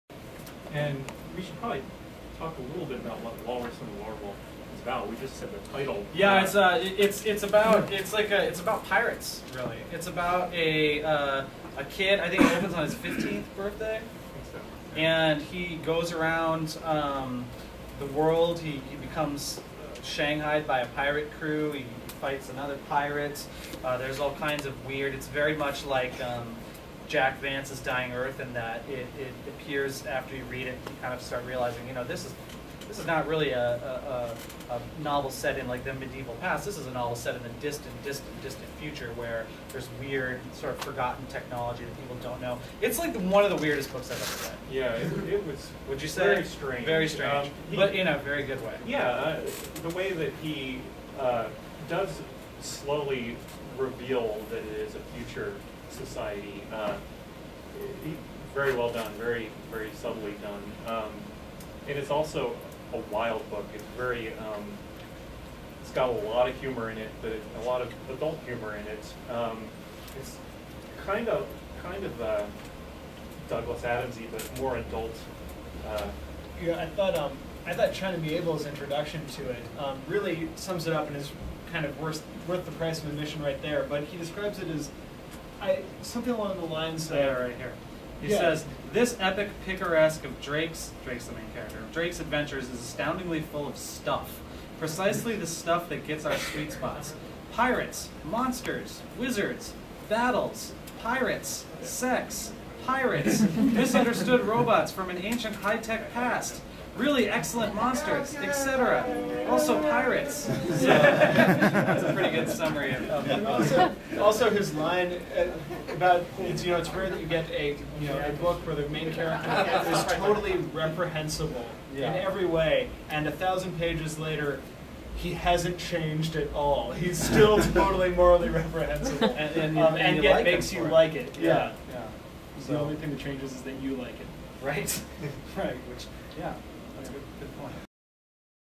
Planet Stories The Walrus & The Warwolf
Pierce Watters, Chris Carey, James Sutter and  Erik Mona discuss The Walrus & The Warwolf. This story by Hugh Cook was a romanticized highlight during the Planet Stories Discussion event. Video taken at PaizoCon '09 in Bellevue Coast Hotel on 12 June 2009, Tamarack Room. Video made with handheld, please excuse sudden shakes.